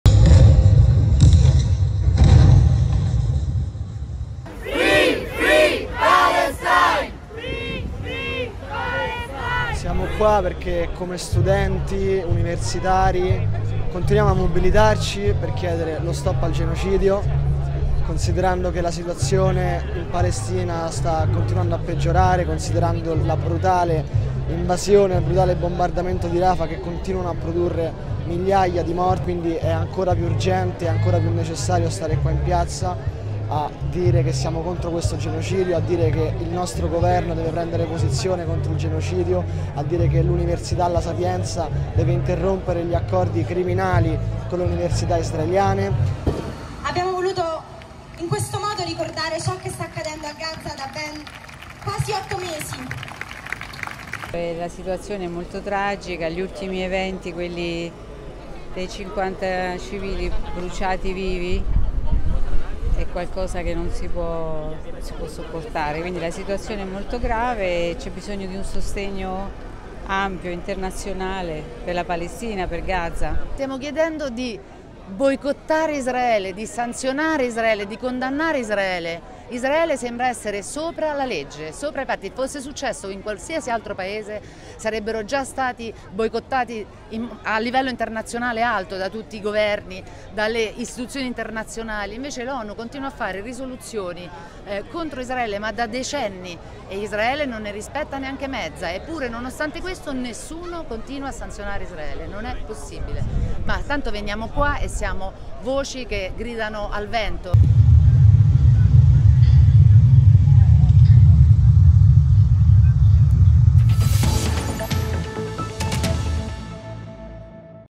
İsrail'in Refah'taki saldırıları Roma'da protesto edildi
İsrail'in Gazze Şeridi'nin güneyindeki Refah kentine yaptığı saldırılar, İtalya'nın başkenti Roma'da protesto edildi